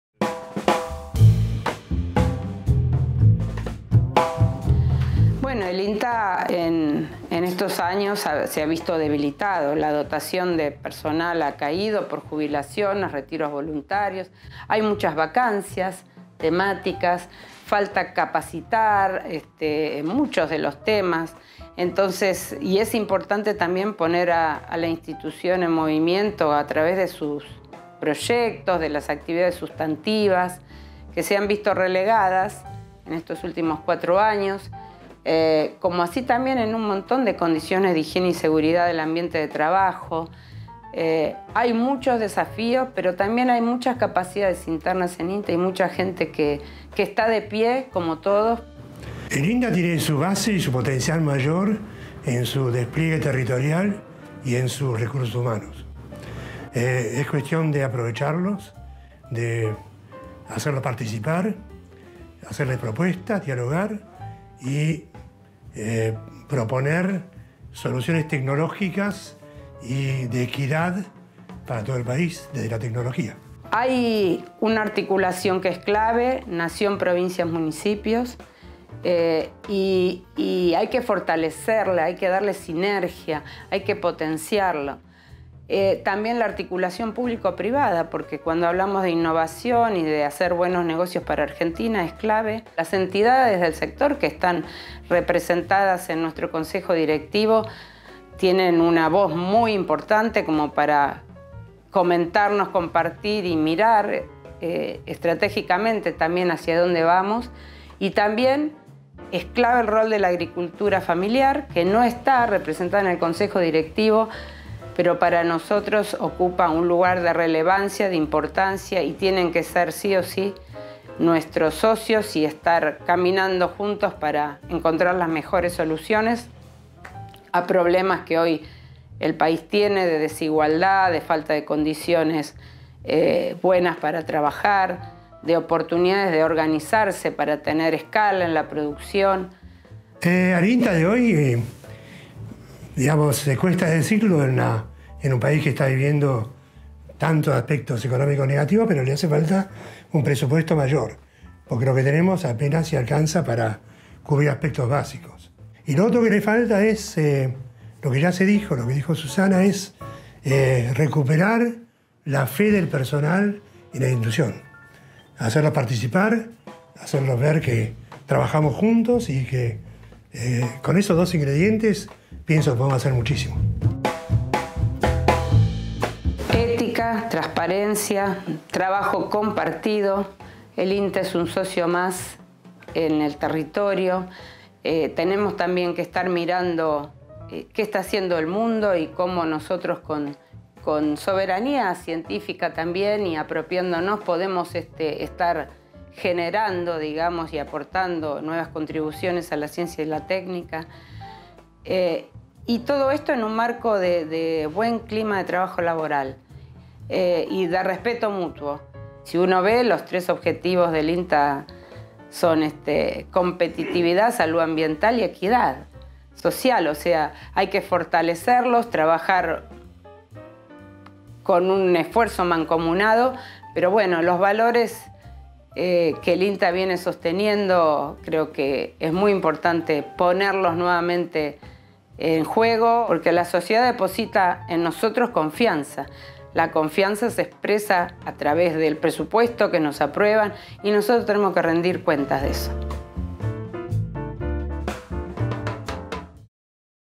Un INTA de pie y de cara a la sociedad
Hablamos con Susana Mirassou y Tomás Schlichter, presidenta y vicepresidente del INTA, sobre cuáles son los ejes priorizados para el organismo en esta nueva etapa.